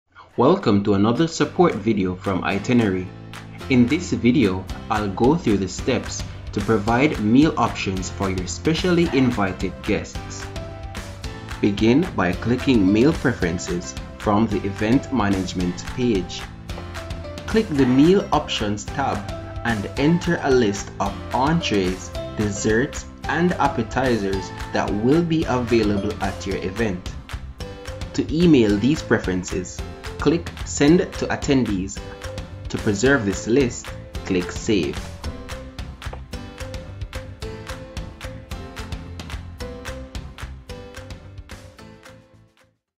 How to Offer Meal Options For Your Event's Guests and Attendees | iTeneri
This video shows you how to offer your guests and attendees catering options with iTeneri.

If you know what kind of food your guests are looking for, you can budget much better and your guests are far more likely to be pleased. So offering meal options prior to your events can be the deciding factor for your event's catering success.

iTeneri is an event management software that makes it easier to plan, manage and host your event.